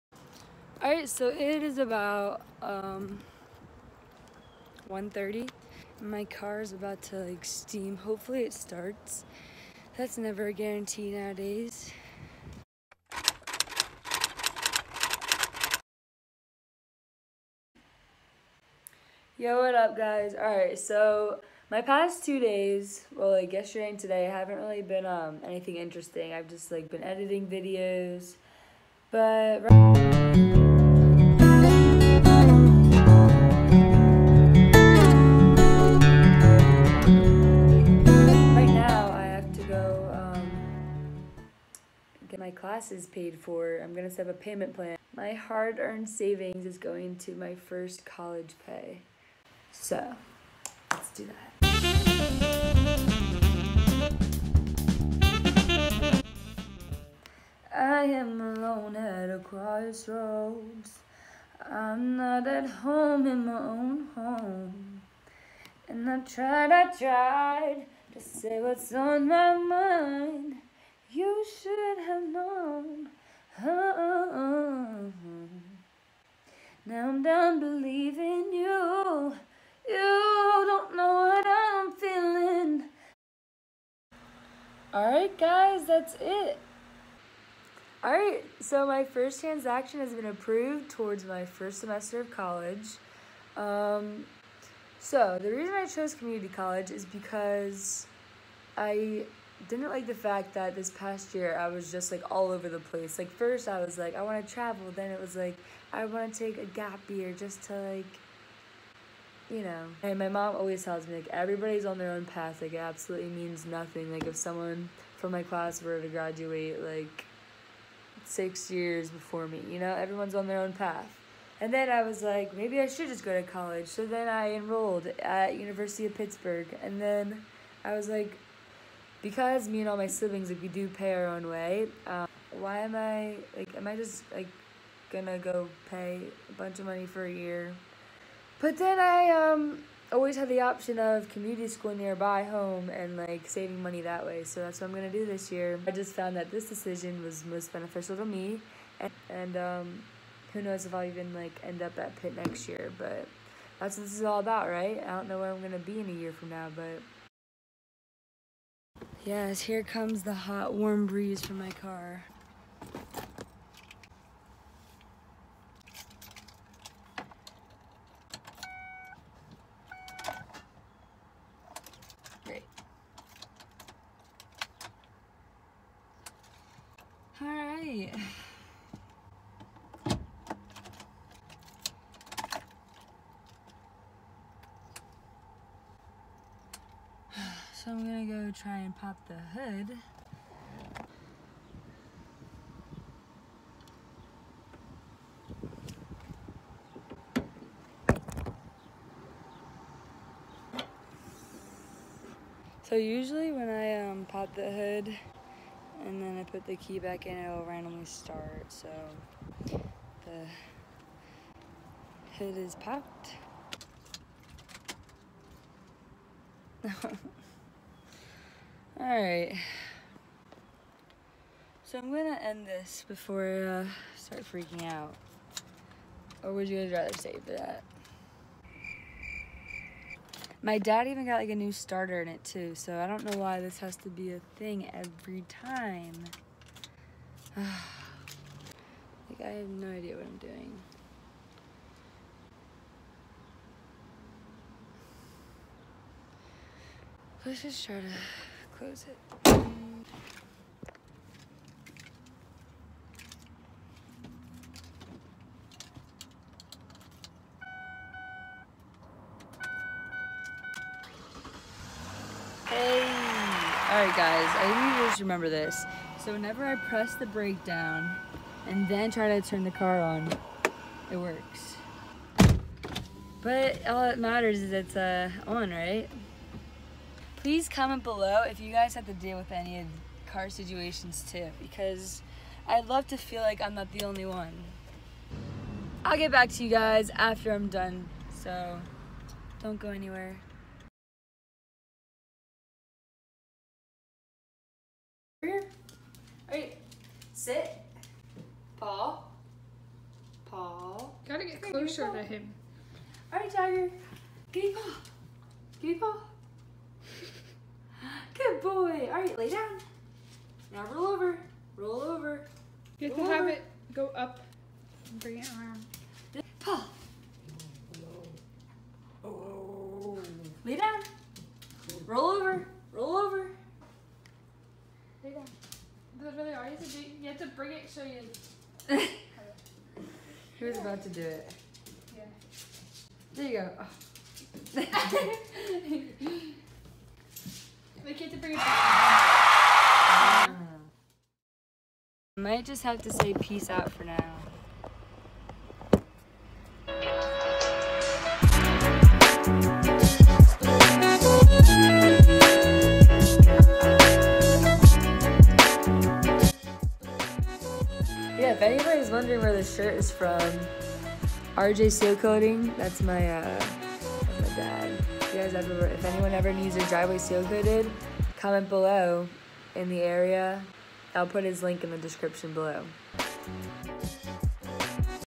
VLOG 4: FIXING CAR BATTERY WITH OUT CABLES
just another day of olivia!! watch the struggle become so real... and learn how to fix a broken car battery without cables